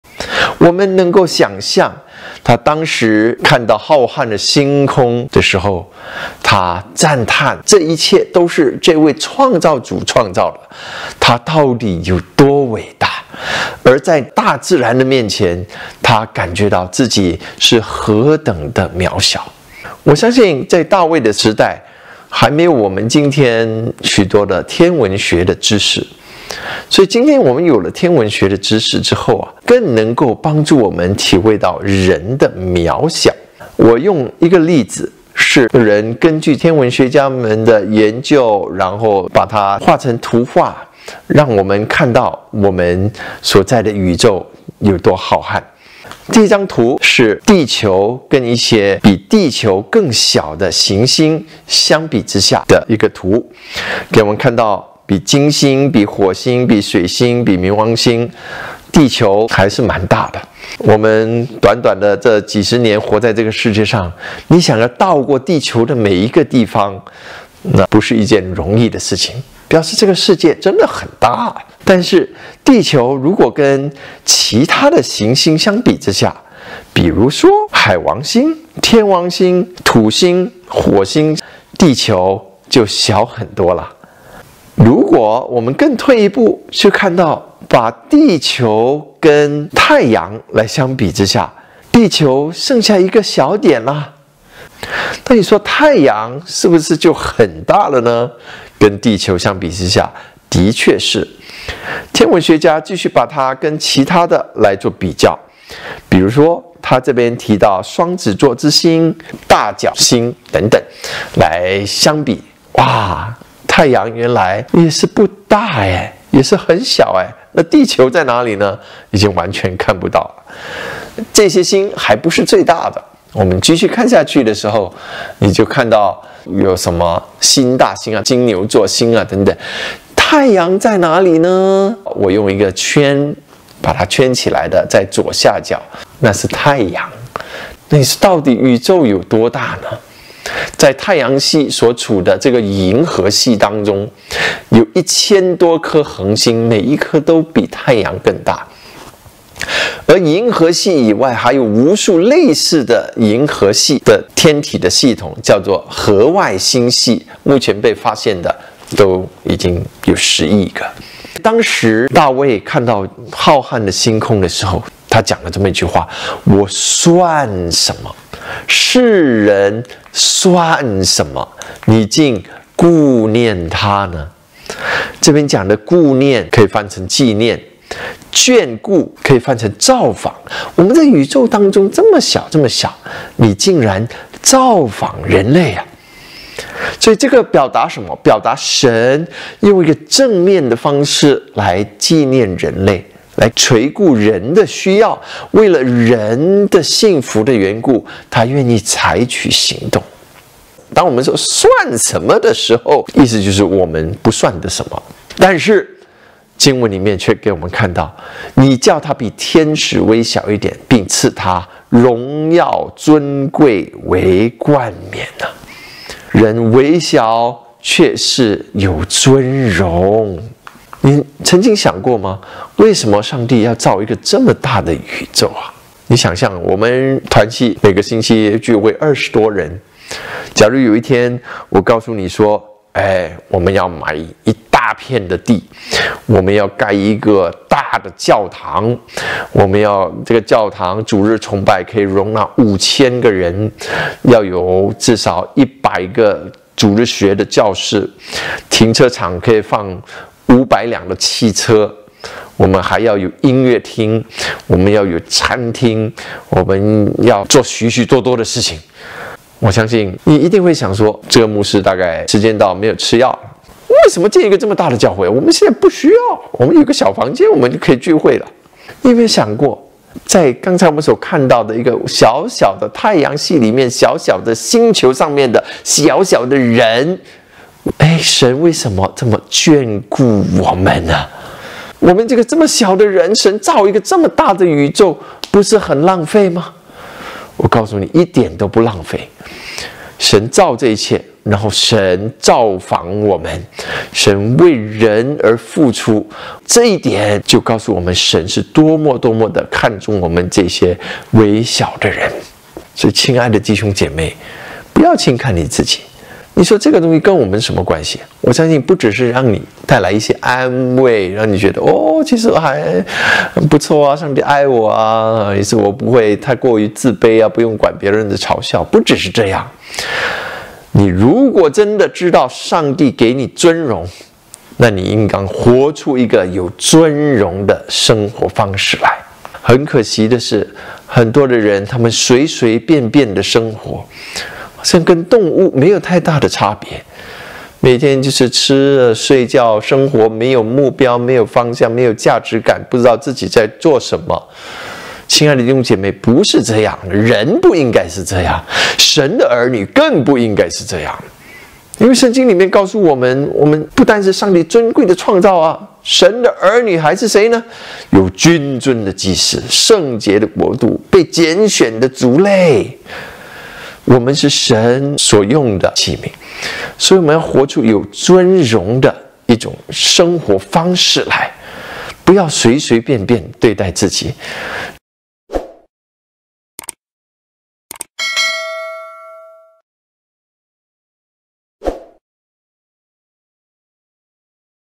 神透過浩瀚宇宙要告訴我們的事……
有沒有想過，為什麼神要造一個這麼大的宇宙？……

蔡頌輝牧師（Andrew Chai）分享